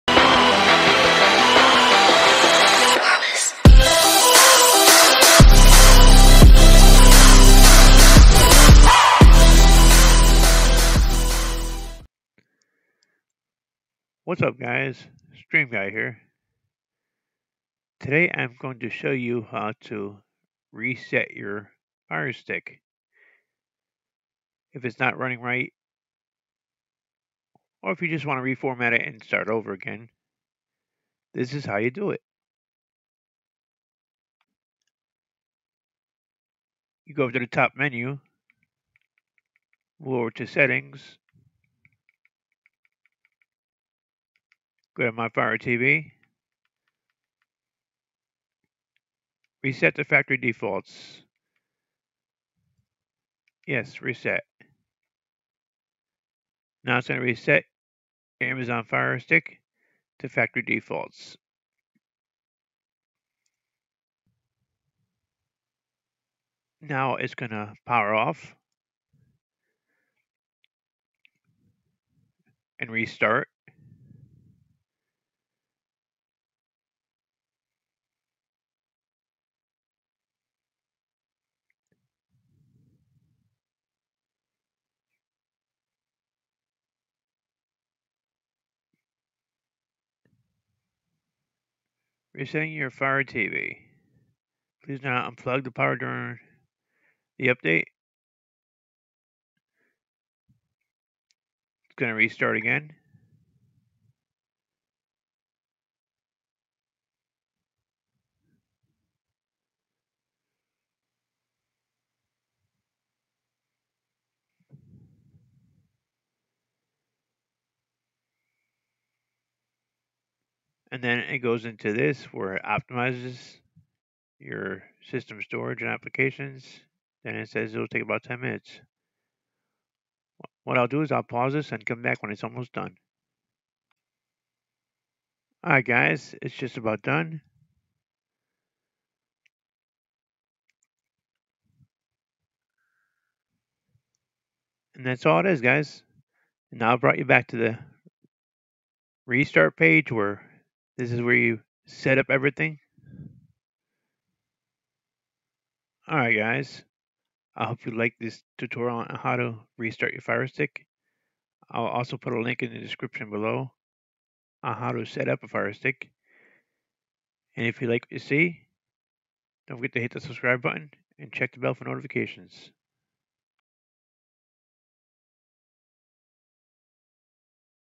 Firestick reset to factory default tutorial
This is a tutorial on how to reset your amazon firestick to factory default.
Firestick lite
Capture card
Gaming Headset